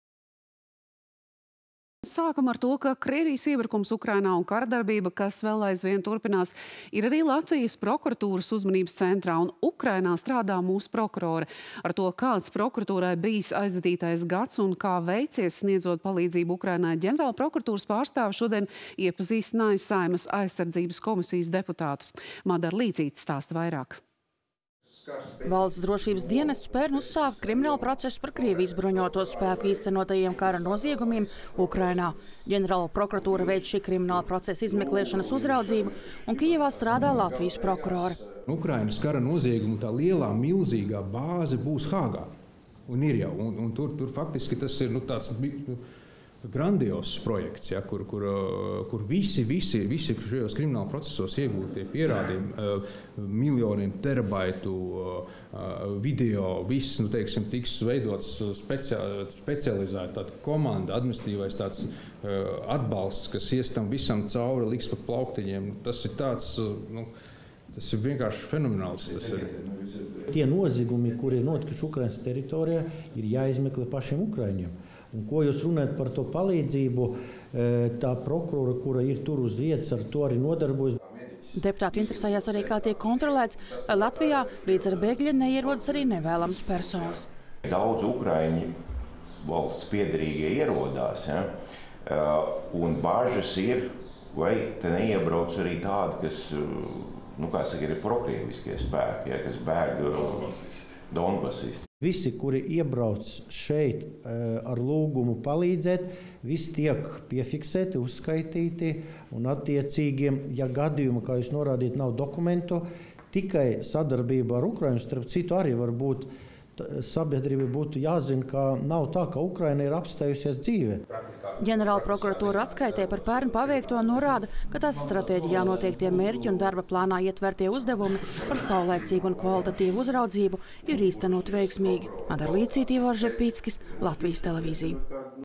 Ģenerālprokurors iepazīstina deputātus ar pērn paveikto
Krievijas iebrukums Ukrainā un karadarbība, kas vēl aizvien turpinās, ir arī Latvijas Prokuratūras uzmanības centrā, un Ukrainā strādā mūsu prokurore. Ar to, kāds prokuratūrai bijis aizvadītais gads un kā veicies, sniedzot palīdzību Ukrainai, Ģenerālprokuratūras pārstāvji šodien iepazīstināja Saeimas Aizsardzības komisijas deputātus.  Turpina Madara Līcīte.